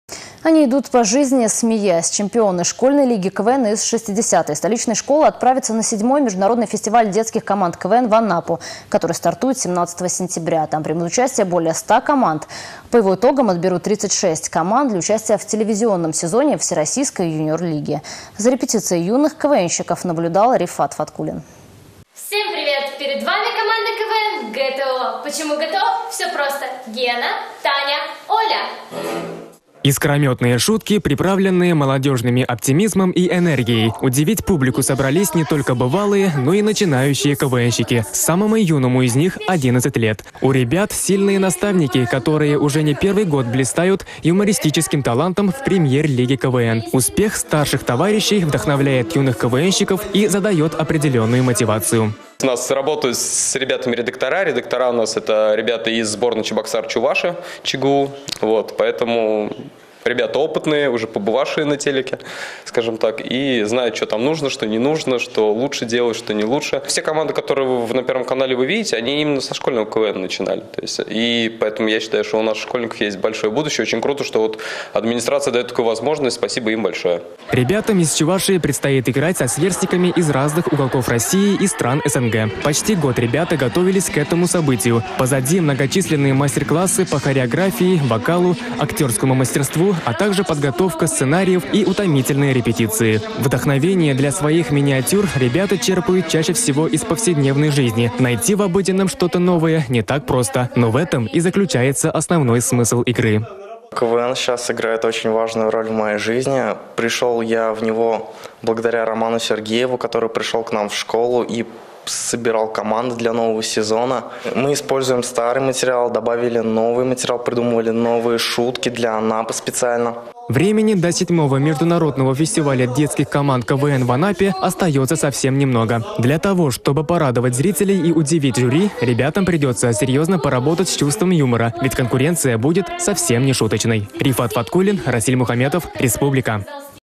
Чемпионы школьной лиги КВН отправятся на 7-ой международный фестиваль детских команд КВН
Они идут по жизни смеясь...Чемпионы школьной лиги КВН из 60 столичной школы отправятся на 7-ой международный фестиваль детских команд КВН в Анапу, который стартует 17 сентября. За репетицией юных квнщиков понаблюдала наша съемочная группа.